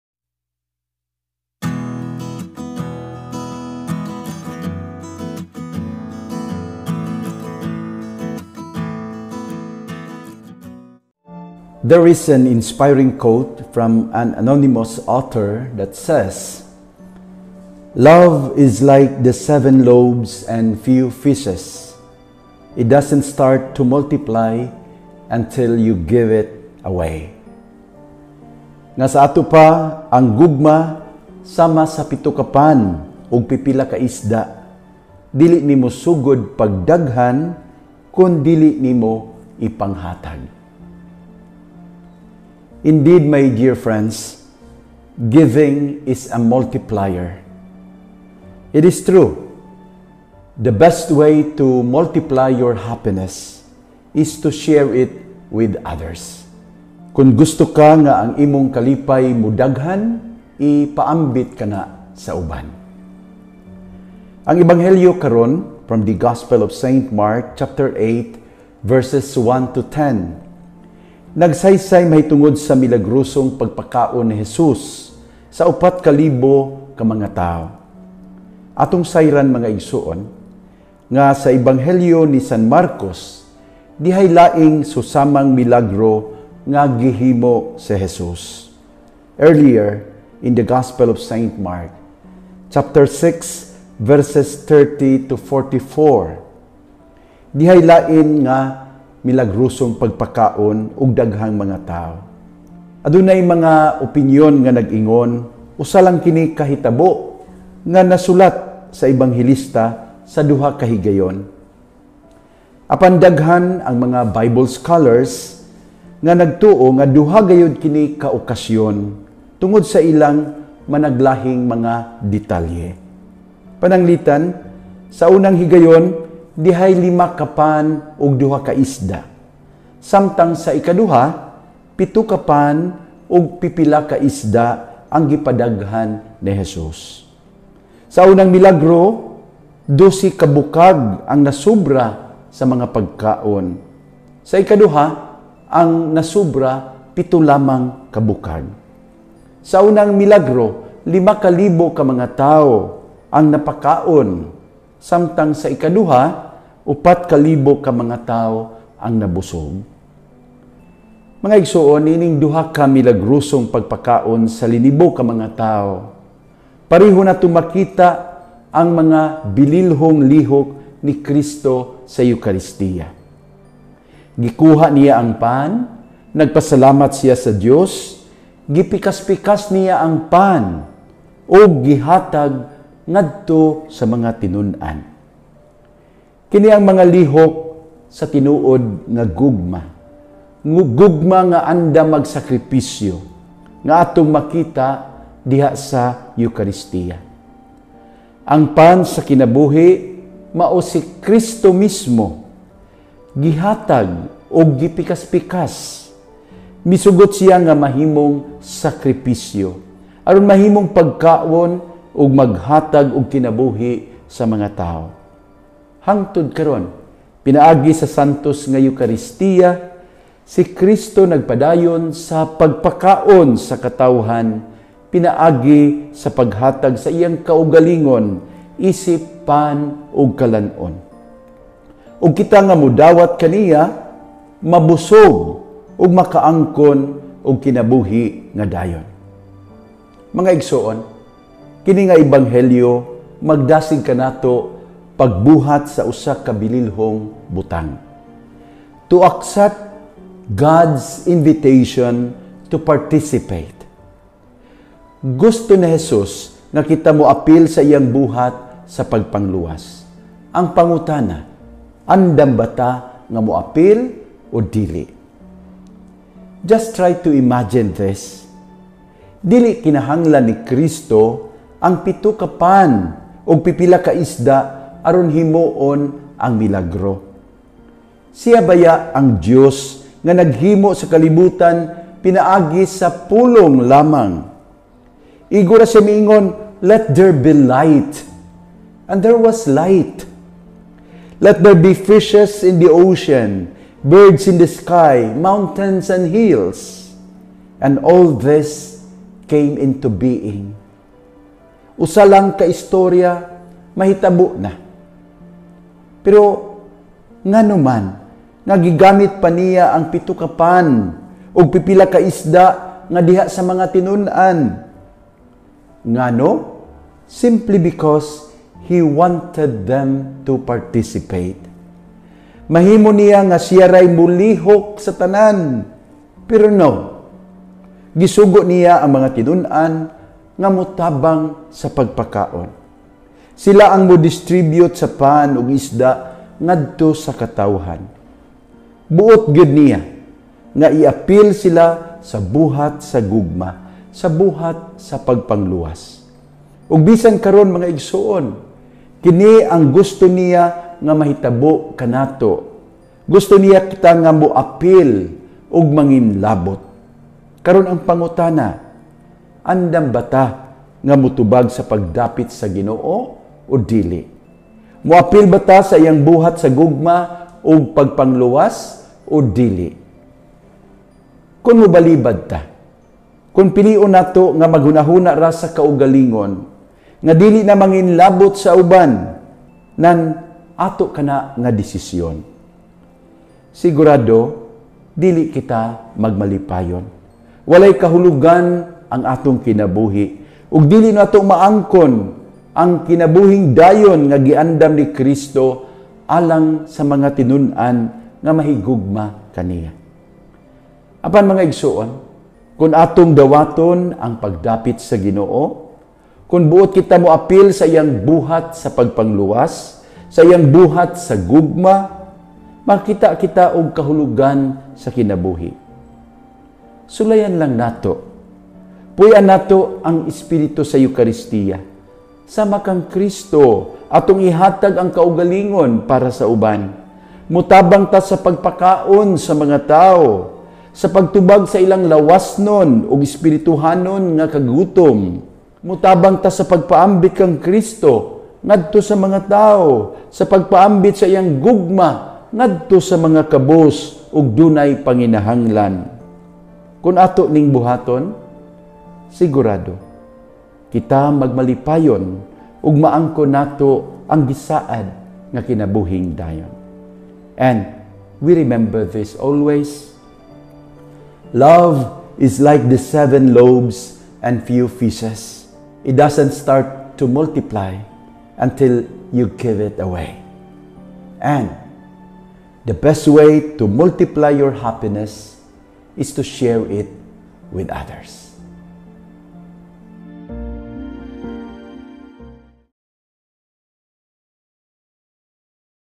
Unsaon pagpadaghan sa atong pagkaon?bishop
- Bishop Abet Uy -
Come & join us! let's hear this very inspiring homily by
- Bishop Abet Uy -
 - Bishop Abet Uy - .